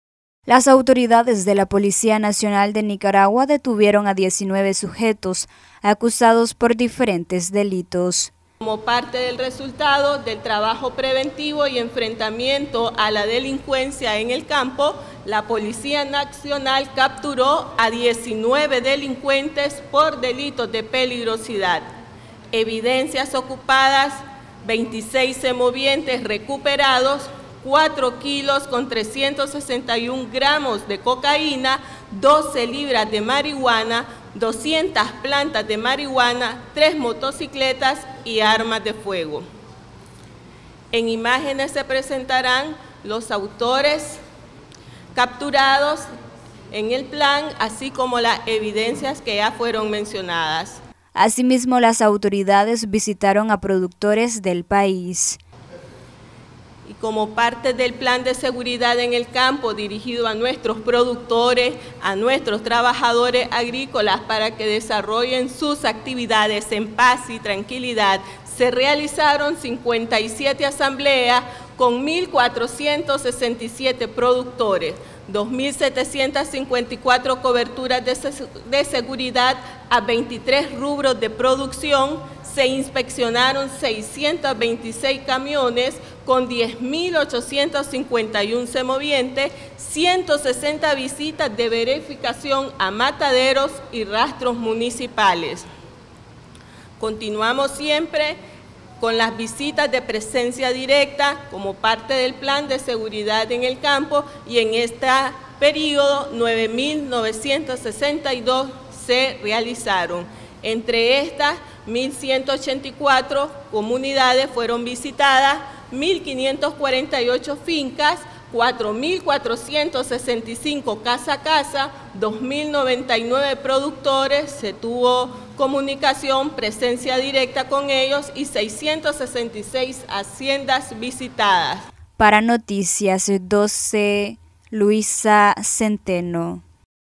Noticias12Nicaragua Detienen a 19 sujetos acusados por diferentes delitos
Detienen a 19 sujetos acusados por diferentes delitos